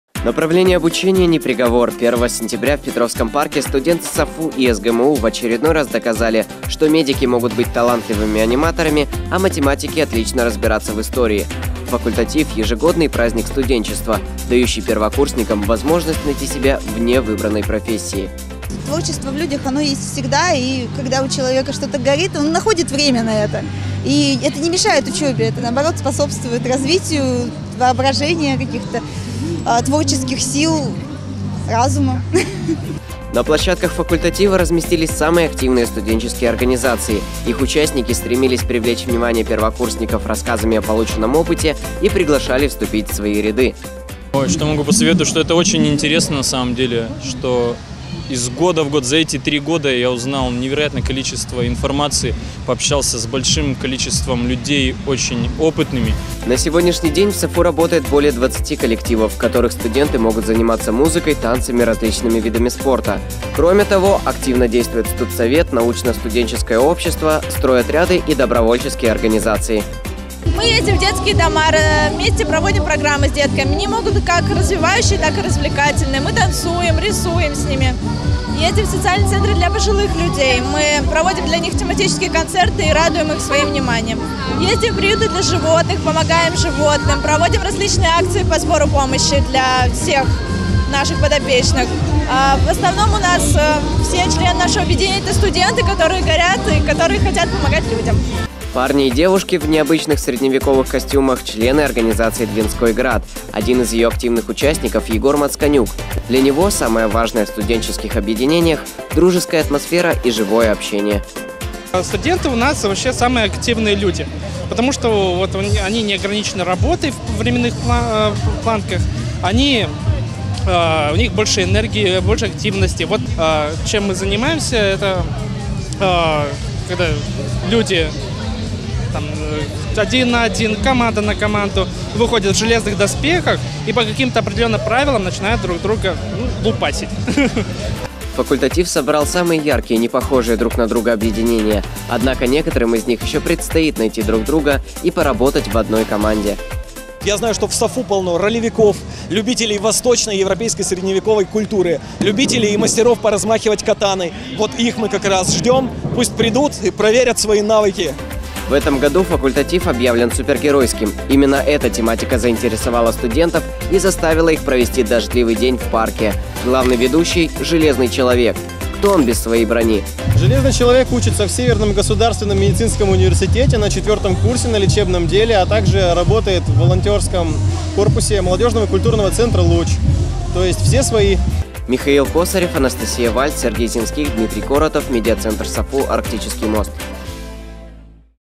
Студенческий праздник Факультатив
Более двадцати университетских объединений были представлены на городском мероприятии, посвященном началу нового учебного года.
Традиционно праздник «Факультатив» проходил в Петровском парке. Для новоиспеченных архангельских студентов выступали музыканты, танцоры, проводились конкурсы и работали тематические площадки, направленные на интеграцию первокурсников в студенческую жизнь.